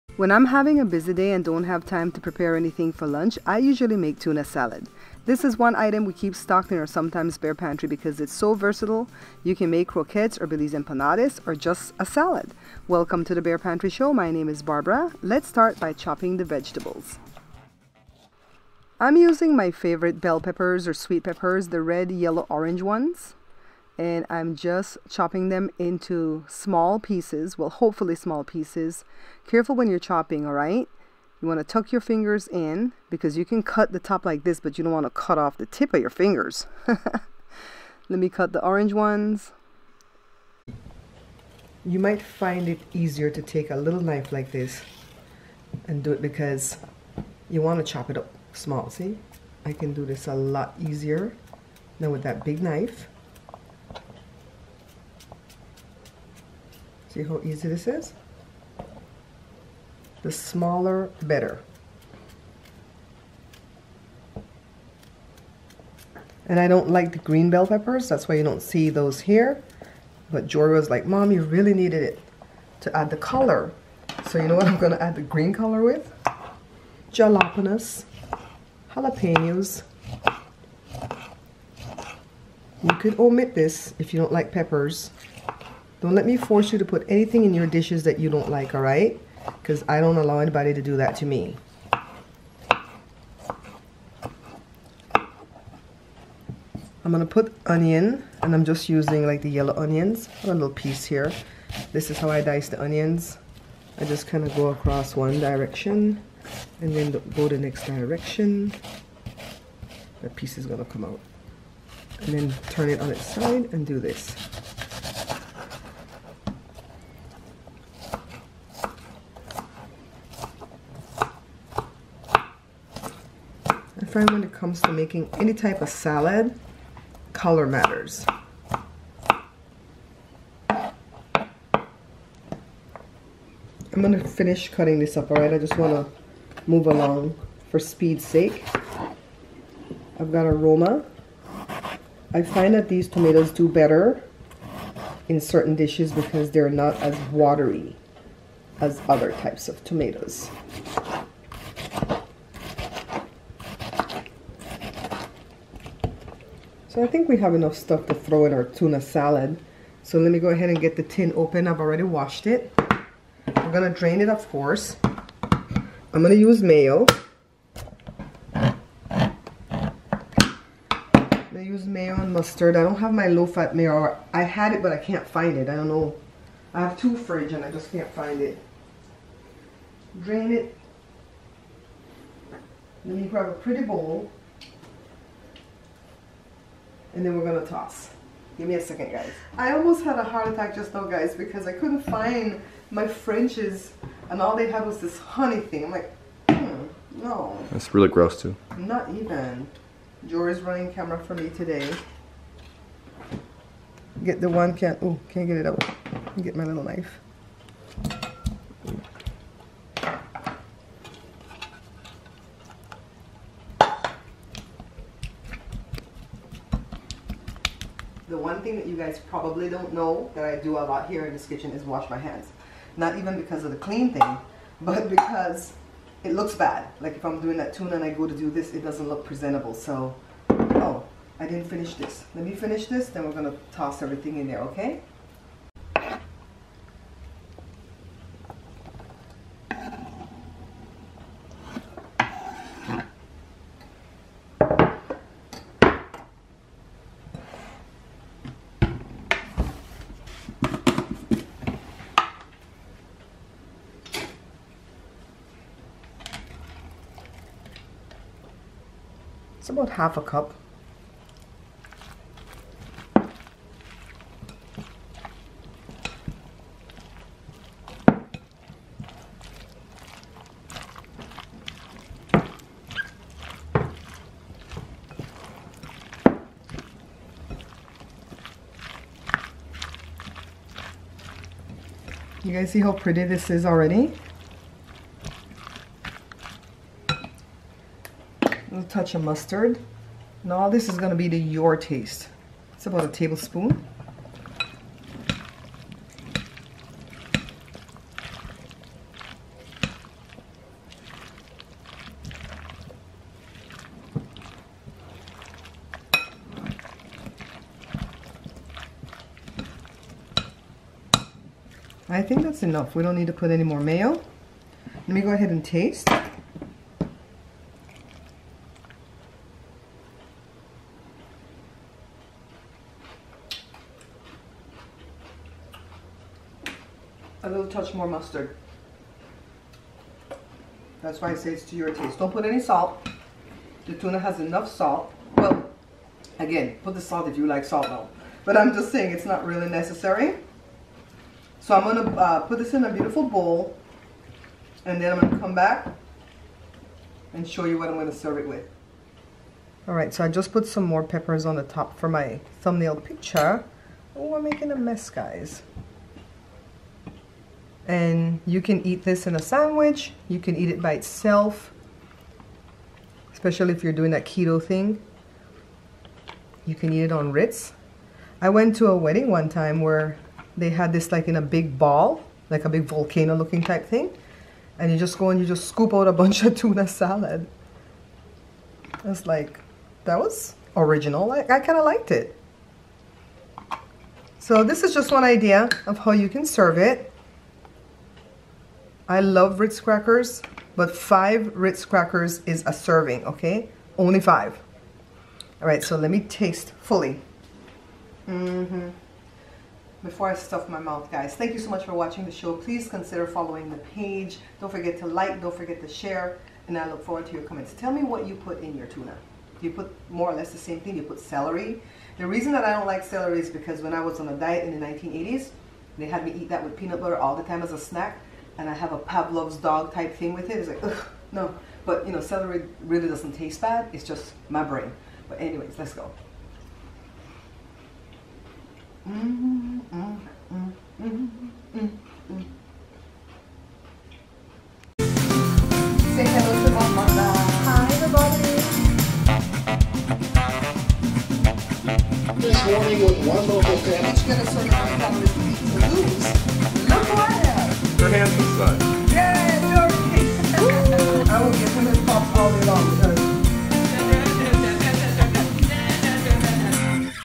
How To Make Tuna Salad
Whenever, I have a busy day and don't have anything prepared for lunch, I grab a can of tuna and make a delicous salad. I add the vegetables I like along with mayo and mustard to make a fresh and colorful salad that can be eaten alone in a sandwich or on crackers.
Amazon Store